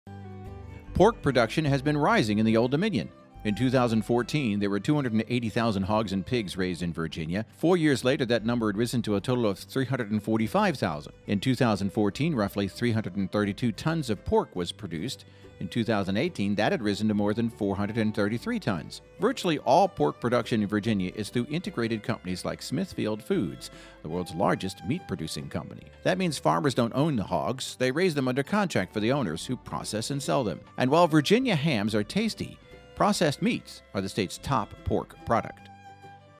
Field to Plate - Pork production rising in Virginia
Find out how much pork is produced in Virginia and what the trends suggest for the future.